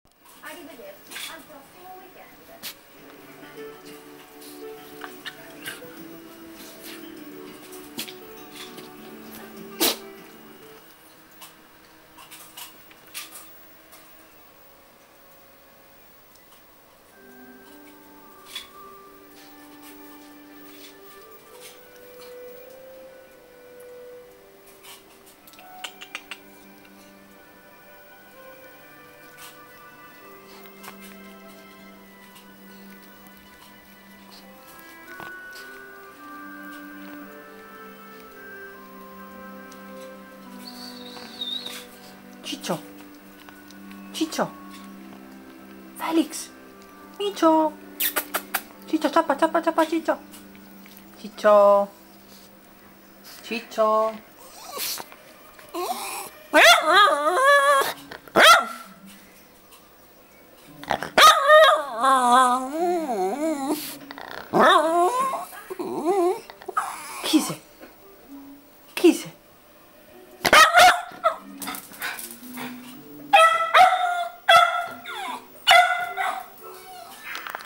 Carlotta - La mia strana cagnolina
La mia cagnolina è stranissima. Non solo si mangia le unghie, ma se le nomini il gatto fa versi stranissimi ihih